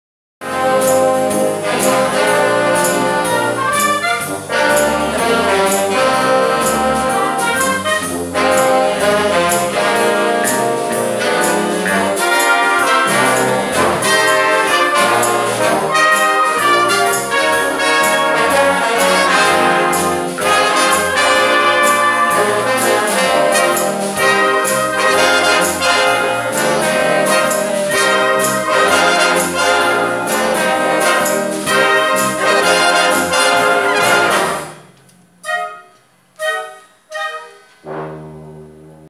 Banda de Música 5 de Agosto
Banda de Música 5 de Agosto - apresentação no Centro Cultural Ariano Suassuna do Tribunal de Contas do Estado da Paraíba,  João Pessoa - PB - 25/03/17.